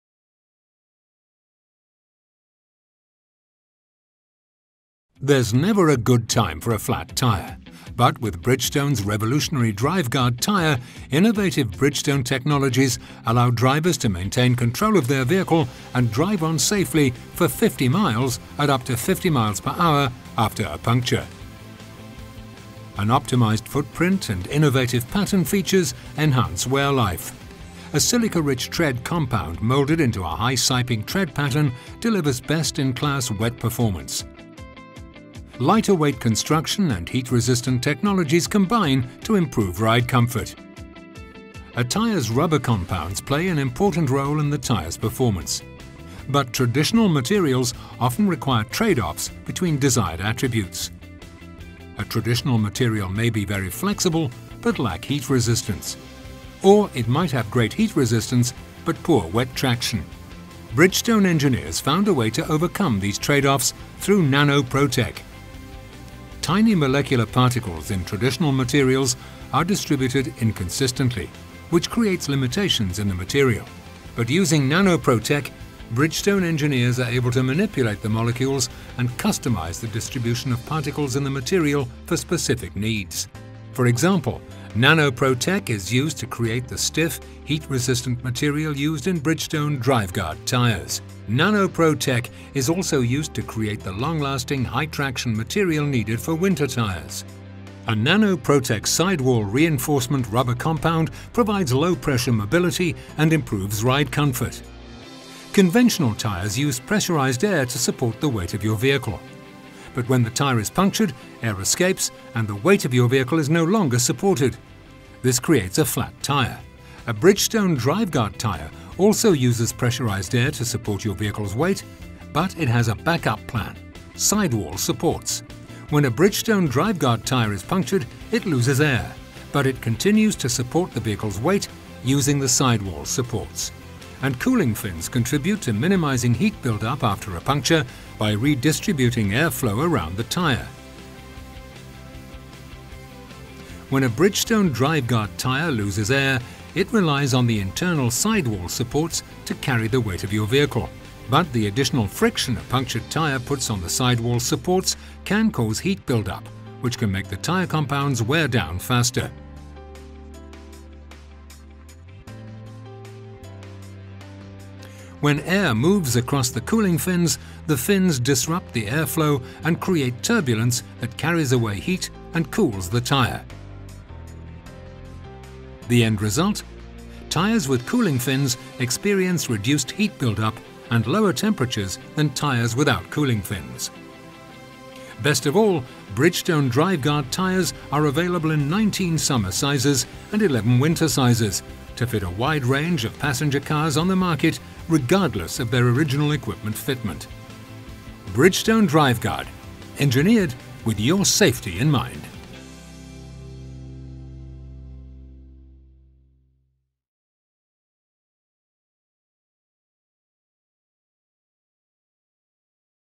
Bridgestone DriveGuard Technology
The revolutionary DriveGuard Tyre with innovative Bridgestone technologies, allowing the driver to maintain control of their vehicle after a puncture and drive on safely for 50 miles, up to 50mph. Technology explained in this video.